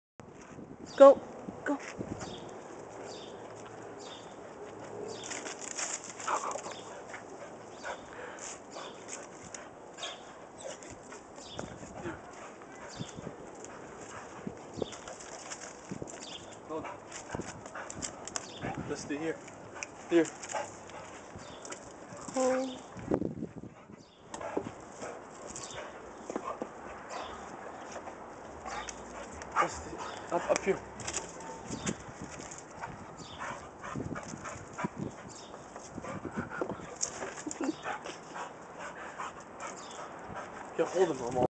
ambitious pitbull
Pitbulls will do anything to get what they want
Good job Dustin!